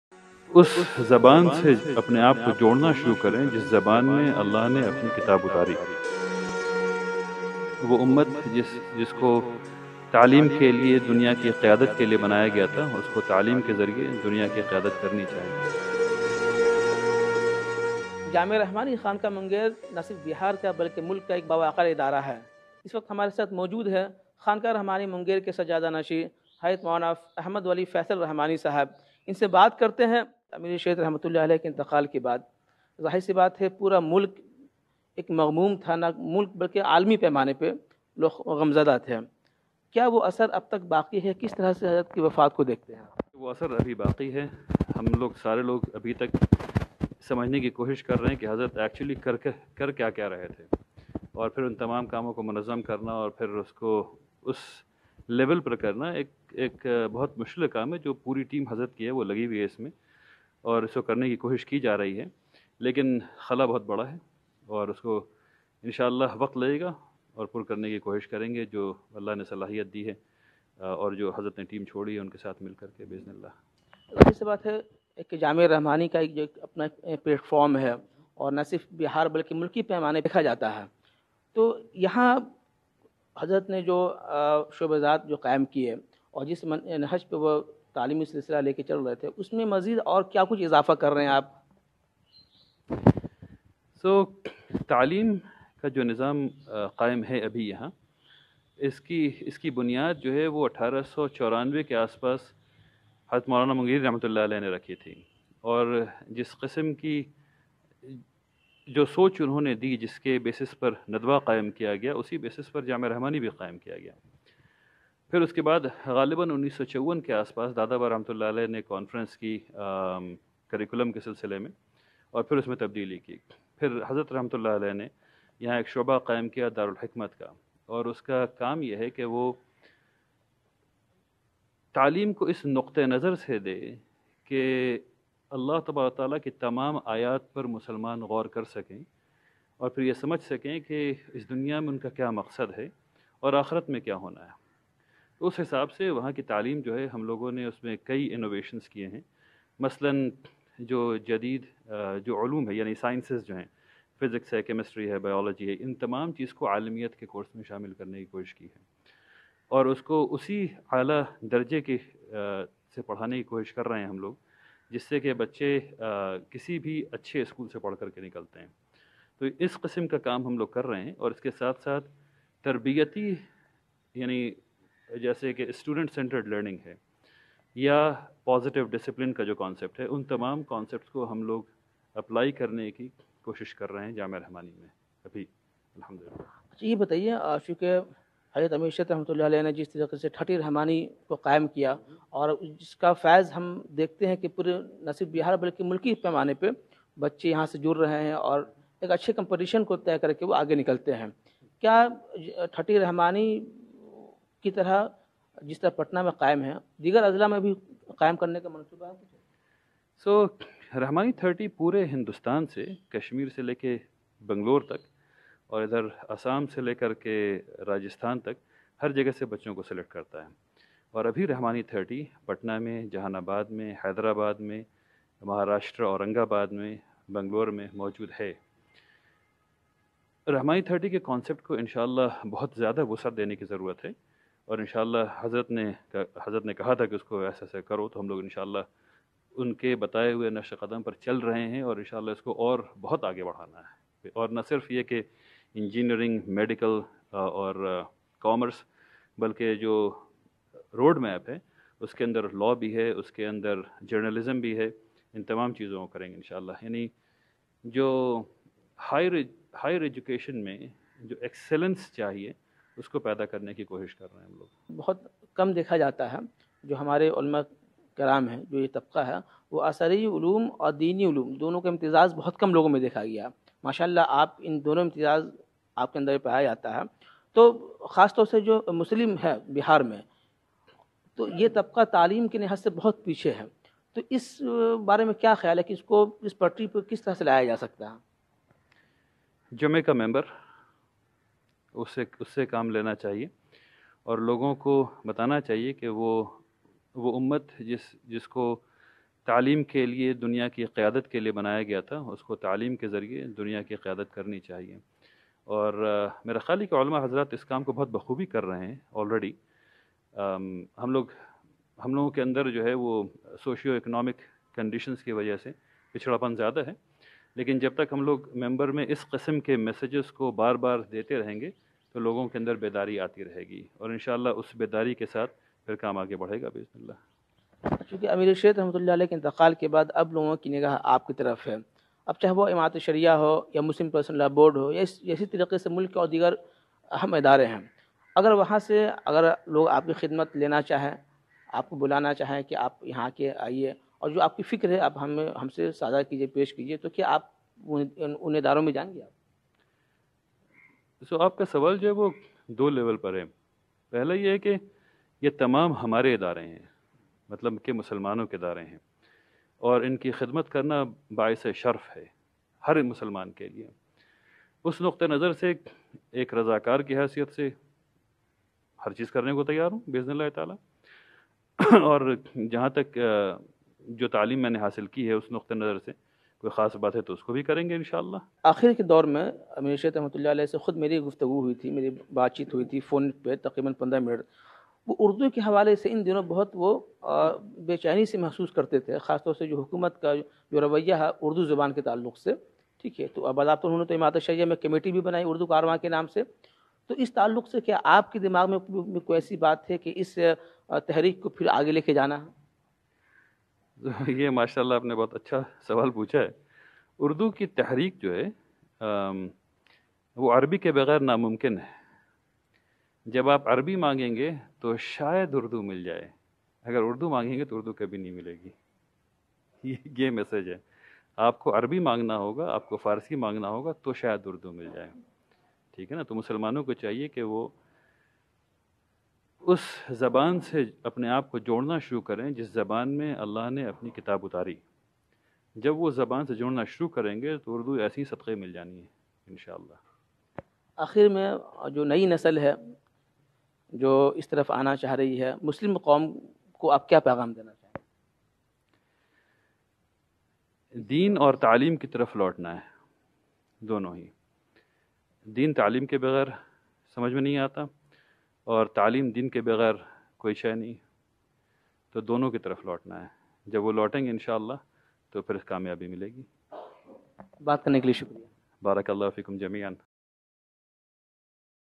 conversation with maulana ahmad wali faisal rahmani
ہم اور آپ جس عہد میں ہیں یہ زمانہ تعلیم کا ہے، حالانکہ اللہ پاک نے تعلیم کی فرضیت اور لازمیت کو آج سے چودہ سو سال قبل ہی واضح کردیا تھا مگر ہم اس پر پوری طرح سے عمل پیرا نہ ہو سکے۔ آج قومِ مسلم زوال کے دہانے پر ہے تو اس کی بنیادی وجہ تعلیم سے دوری ہے، مگر الحمدللہ اب صورتحال پہلے سے قدرے بہتر ہے۔